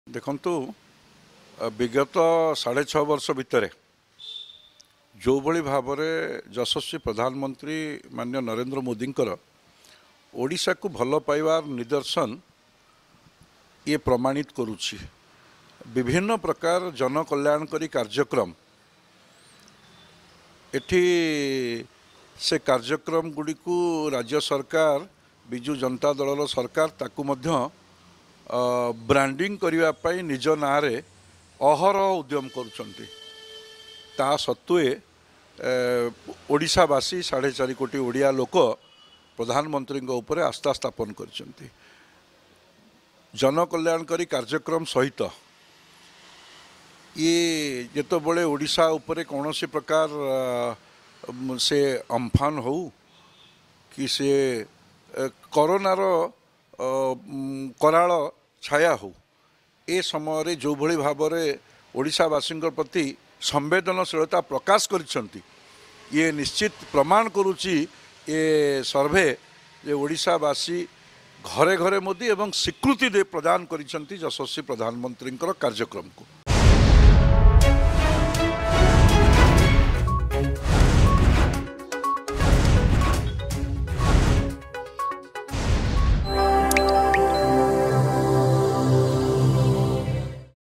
ମୋଦୀଙ୍କ ଉପରେ ସାଢେ 4କୋଟି ଓଡିଶା ବାସୀଙ୍କ ଆସ୍ଥା ରହିଛି
ପ୍ରଧାନମନ୍ତ୍ରୀ ନରେନ୍ଦ୍ର ମୋଦୀଙ୍କ ଉପରେ ସାଢେ 4କୋଟି ଓଡିଶା ବାସୀଙ୍କ ଆସ୍ଥା ରହିଛି.